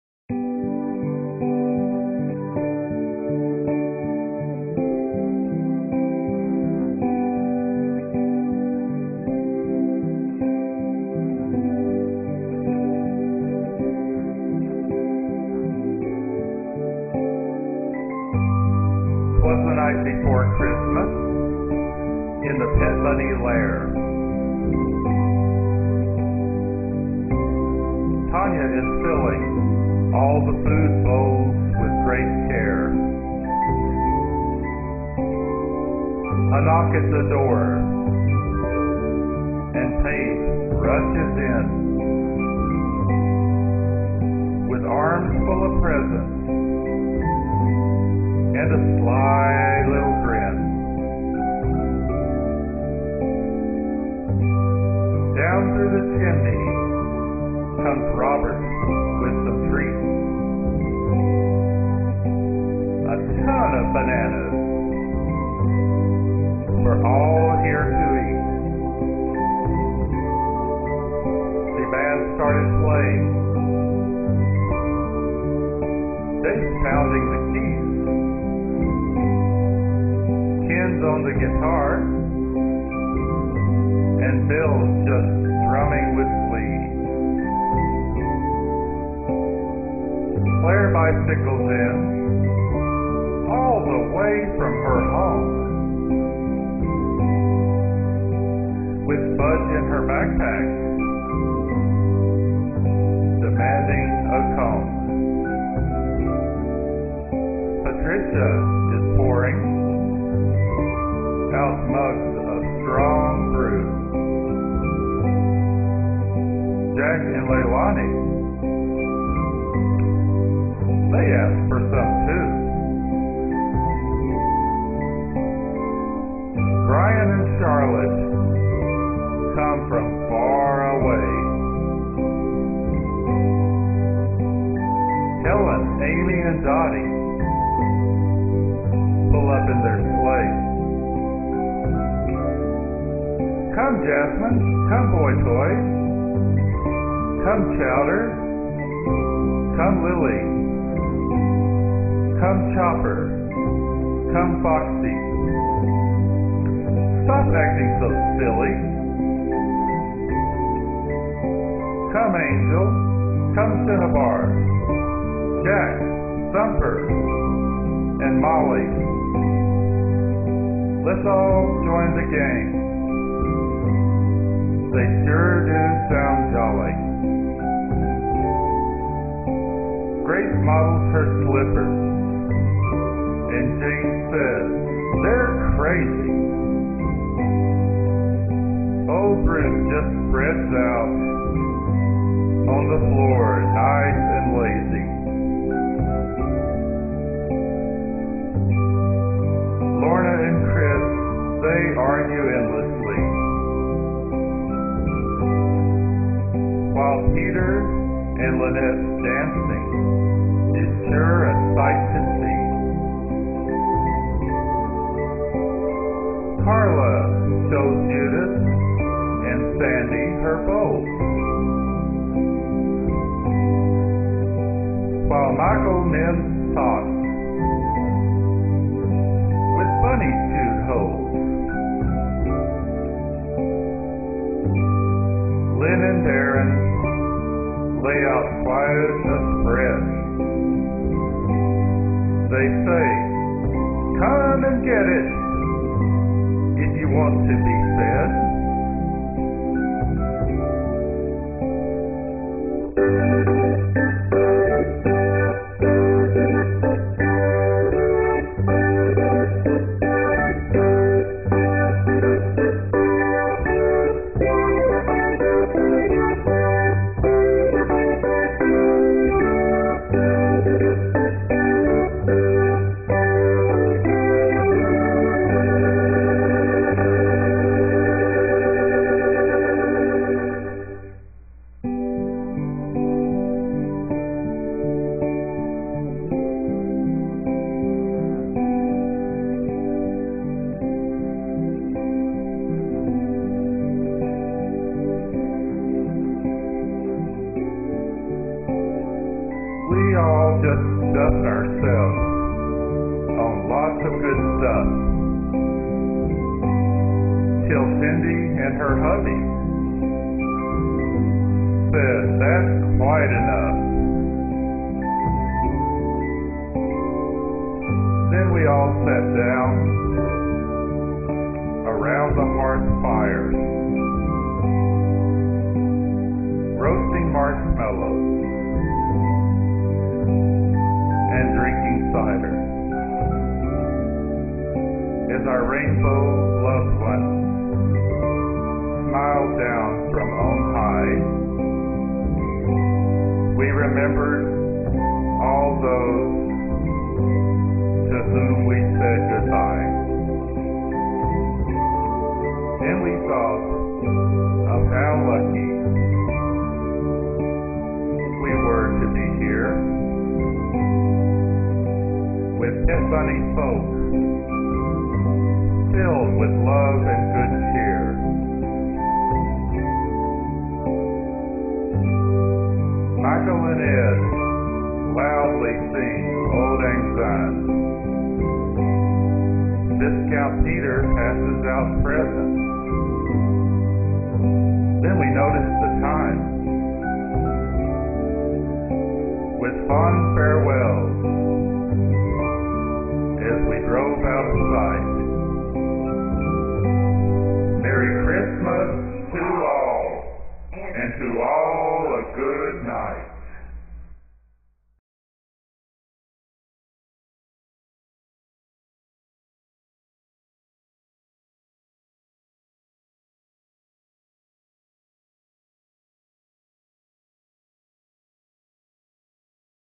Pet Bunny 2008 Christmas
Our internet bunny group Pet Bunny has a virtual Christmas party!